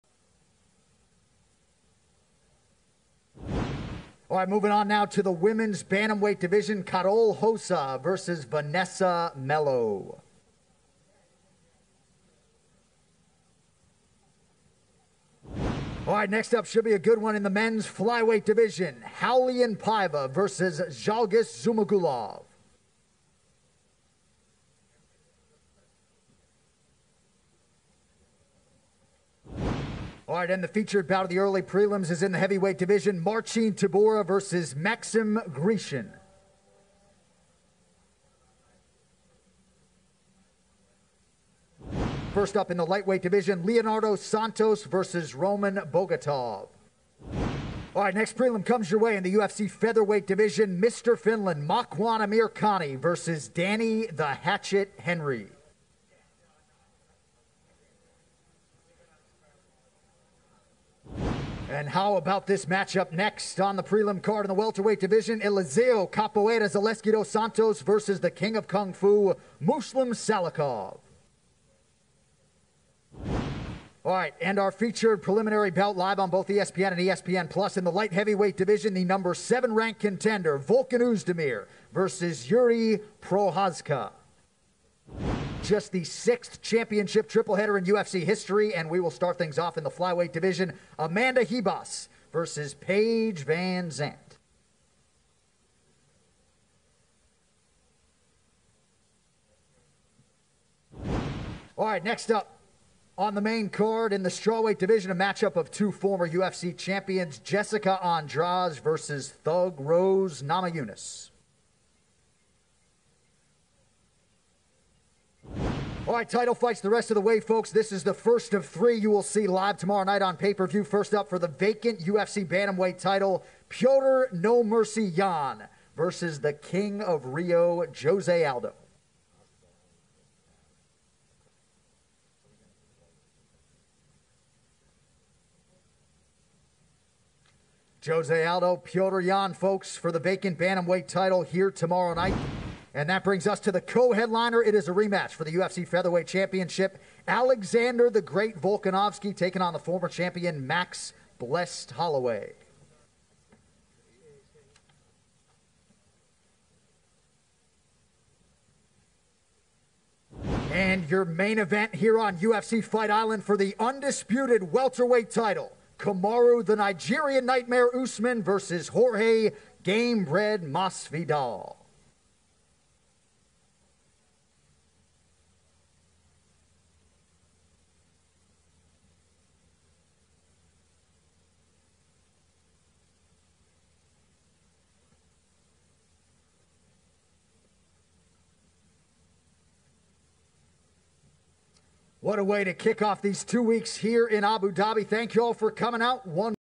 UFC 251 weigh in!! Usman, Masvidal, Vanzant, Rose, Aldo
Weigh ins are here for ufc 251